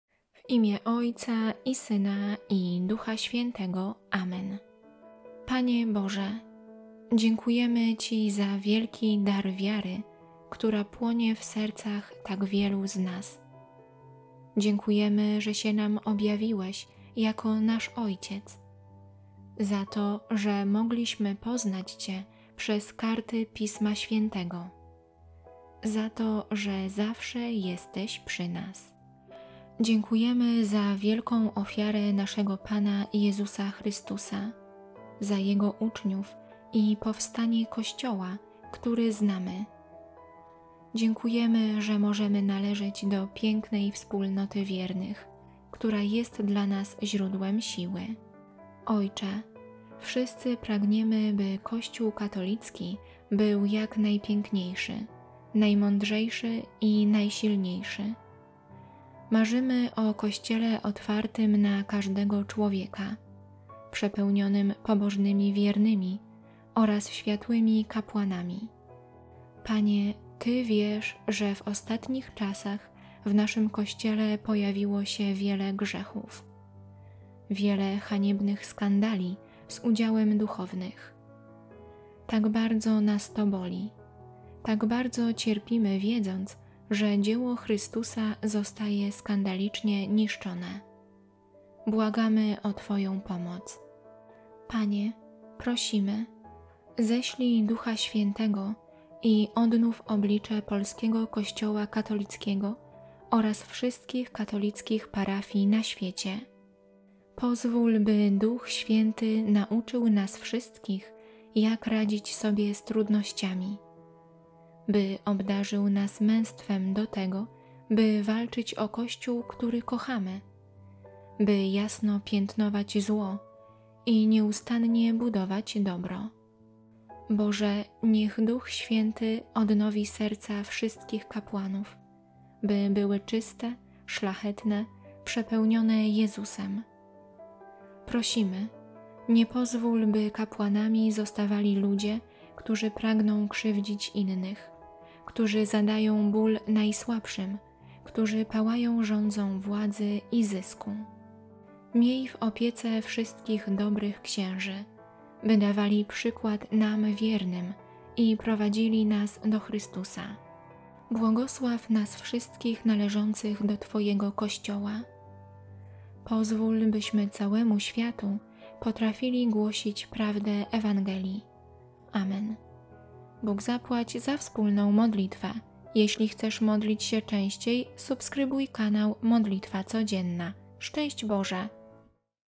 MODLITWA O UZDROWIENIE KOŚCIOŁA Z GRZECHÓW | Modlitwa za grzechy w Kościele Katolickim
MODLITWA O UZDROWIENIE KOŚCIOŁA Z GRZECHÓW to pomocna i potrzebna modlitwa za grzechy w Kościele Katolickim, przeznaczona do prywatnego odmawiania przez osoby, którym leży na sercu dobro wspólnoty.

Panie Boże! Dziękujemy Ci wielki dar wiary, która płonie w sercach tak wielu z nas. Dziękujemy, że się nam objawiłeś jako nasz Ojciec, za to, że mogliśmy poznać Cię przez karty Pisma Świętego, za to, że zawsze jesteś przy nas. Dziękujemy za wielką ofiarę naszego Pana Jezusa Chrystusa, za Jego uczniów i powstanie Kościoła, który znamy. Dziękujemy, że możemy należeć do pięknej wspólnoty wiernych, która jest dla nas źródłem siły.
Ojcze! Wszyscy pragniemy by Kościół Katolicki był jak najpiękniejszy, najmądrzejszy i najsilniejszy. Marzymy o Kościele otwartym na każdego człowieka, przepełnionym pobożnymi wiernymi oraz światłymi kapłanami. Panie, Ty wiesz, że w ostatnich czasach w naszym Kościele pojawiło się wiele grzechów, wiele haniebnych skandali z udziałem duchownych. Tak bardzo nas to boli, tak bardzo cierpimy wiedząc, że dzieło Chrystusa zostaje skandalicznie niszczone. Błagamy o Twoją pomoc.
Panie, prosimy, ześlij Ducha Świętego i odnów oblicze Polskiego Kościoła Katolickiego oraz wszystkich katolickich parafii na świecie. Pozwól, by Duch Święty nauczył nas wszystkich jak radzić sobie z trudnościami, by obdarzył nas męstwem do tego, by walczyć o Kościół, który kochamy, by jasno piętnować zło i nieustannie budować dobro.
Boże, niech Duch Święty odnowi serca wszystkich kapłanów, by były czyste, szlachetne, przepełnione Jezusem. Prosimy, nie pozwól, by kapłanami zostawali ludzie, którzy pragną krzywdzić innych, którzy zadają ból najsłabszym, którzy pałają żądzą władzy i zysku. Miej w opiece wszystkich dobrych księży, by dawali przykład nam wiernym i prowadzili nas do Chrystusa. Błogosław nas wszystkich należących do Twojego Kościoła, pozwól, byśmy całemu światu potrafili głosić prawdę Ewangelii. Amen.